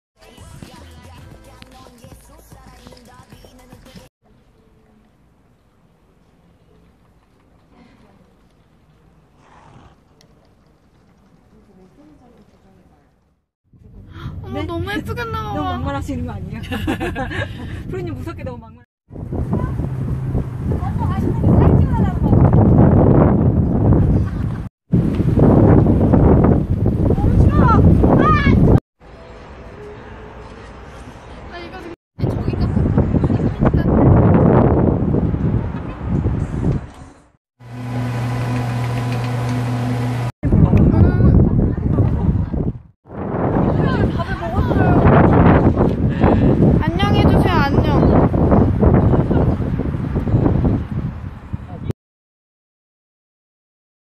[HAYOU's Vlog] 회사 점심시간에 한강가기
회사만 나왔다하면 웃음꽃이 만개하는
사람 직장인ㅋㅋ

점심시간 2시간인 도시락데이에
한강으로 팀원들과 고고!!:)

뽀글이 라면도 먹고 노래틀고
일광욕도 즐기고..역시 회사밖은 즐거워♡

한강 나들이로 신난 직장인을
감상(?)해보세요ㅋㅋ